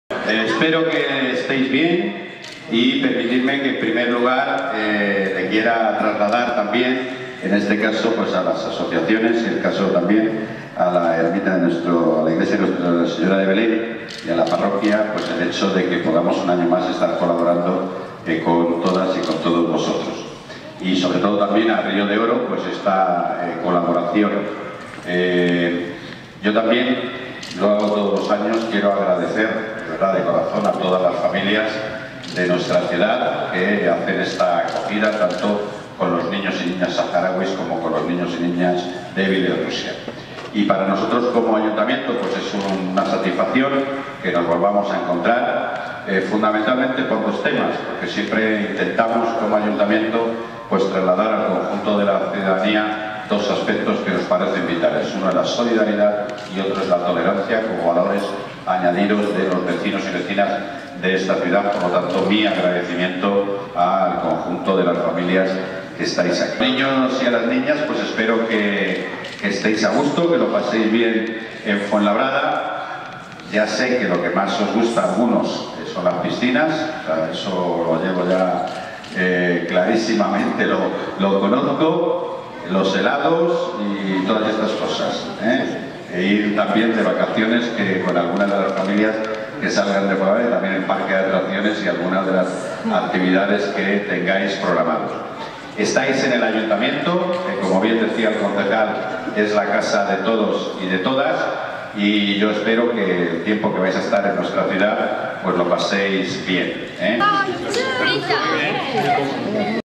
El Alcalde recibe a los niños bielorrusos y saharauis que hoy visitan el Ayuntamiento
El alcalde les ha dado la bienvenida a la ciudad y ha destacado la solidaridad y generosidad de los artífices del proyecto y de los participantes.

El alcalde, Manuel Robles, ha recibido esta mañana a los 34 niños saharauis y bielorrusos que pasan sus días de vacaciones acogidos por familias fuenlabreñas.

El alcalde les ha dado la bienvenida a la ciudad en el salón de Plenos, donde les ha entregado algunos obsequios y les ha ofrecido datos generales de la ciudad

Sobre todo, Manuel Robles, ha destacado la solidaridad y generosidad de las familias acogedores que no dudan en incluir en la intimidad de sus hogares a estos niños que proceden de lugares en los que viven en duras condiciones.

“Los saharauis proceden de los campos refugiados de Tinduf donde soportan duras condiciones meteorológicas y los bielorrusos vienen de un entorno contaminado por el desastre nuclear de Chernóbil”, apunta el alcalde.

También ha agradecido a los artífices del proyecto su iniciativa, tanto a la Asociación Río de Oro como a la Parroquia Nuestra Señora de Belén, por el esfuerzo que desde hace años realizan, en colaboración con el Ayuntamiento, “para que esta gratificante experiencia sea una realidad”.

Este año son 14 los niños saharauis y una veintena de bielorrusos, con edades comprendidas entre lo 9 y 14 años, los que pasarán sus vacaciones estivales en la ciudad.

Durante su estancia los niños serán sometidos a revisiones médicas en el Hospital de Fuenlabrada y odontológicas en la Universidad Rey Juan Carlos (URJC), gracias al convenio alcanzado entre las entidades.

Además participarán en actividades lúdicas, culturales y deportivas, realizarán excursiones a puntos de interés y disfrutarán de la piscina municipal. También participarán en encuentros entre las distintas familias acogedoras.

Las familias siempre expresan al final del programa su gratitud a los niños por todo lo que les aportan durante su estancia. “Tienen una mirada diferente y nos hacen valorar cosas cotidianas que nos pasan desapercibidas en el día a día. Además la convivencia con nuestros hijos siempre es muy enriquecedora para ambas partes”.